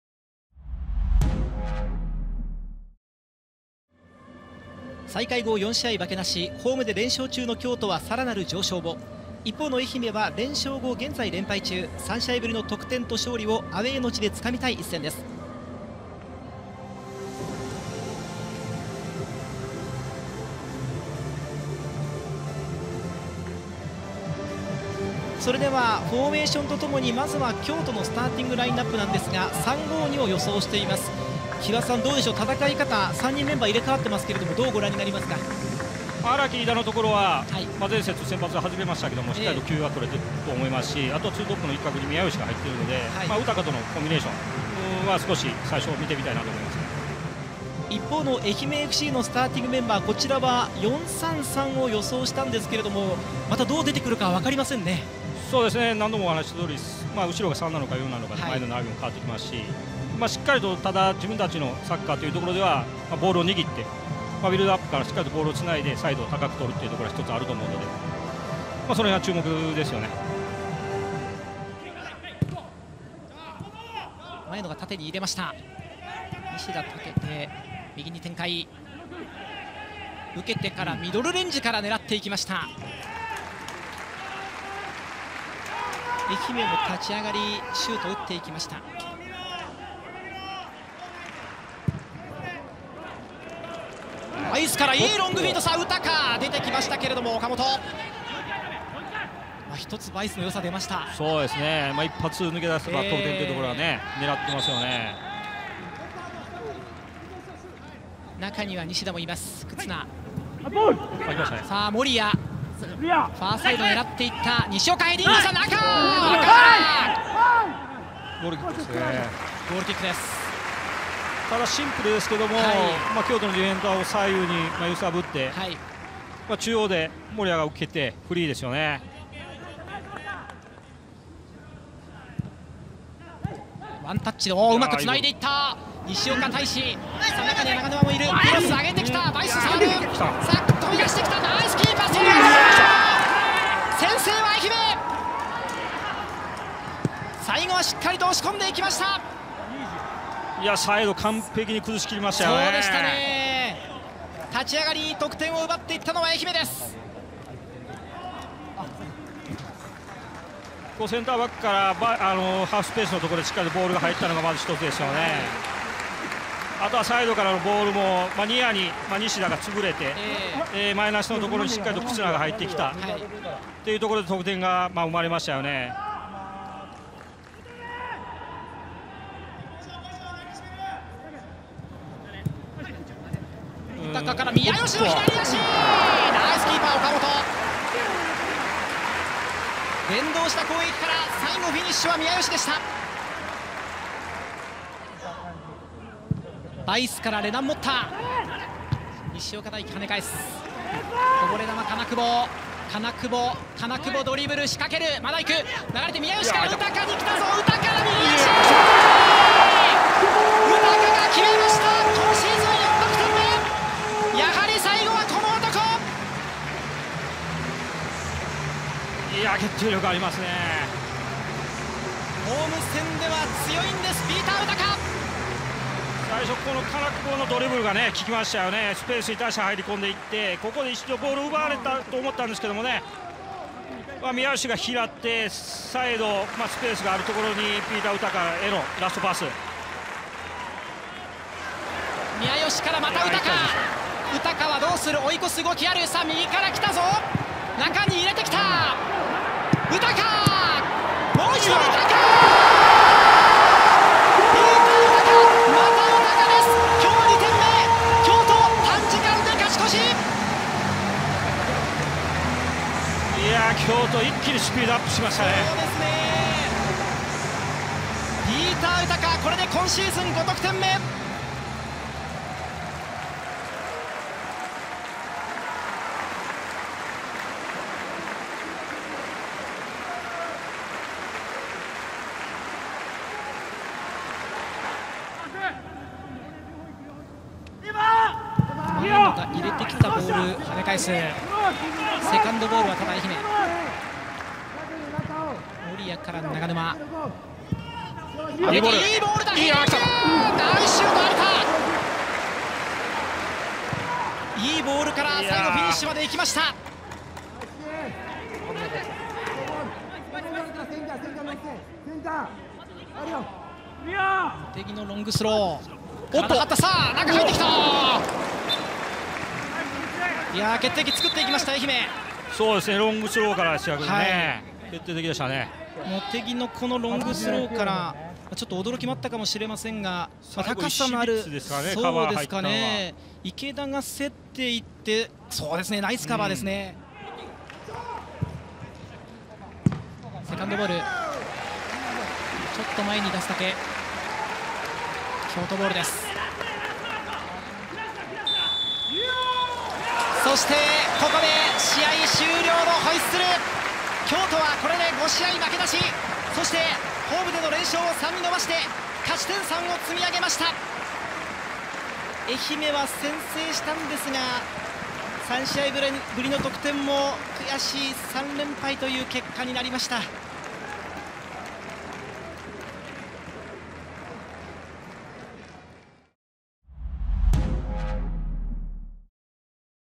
【ハイライト】2020明治安田生命J2リーグ第6節vs.愛媛@サンガS
2020年7月19日（日）18:33キックオフ
京都サンガF.C. 2-1 愛媛FC
[会場]サンガスタジアム by KYOCERA